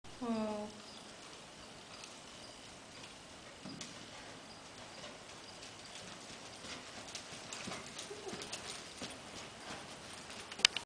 Ms. Toby
Ms. Toby from Saddlebred Rescue - I had the pleasure of taking her for a test ride at her foster home in Elgin, IL.  Ms. Toby needs a forever home!!!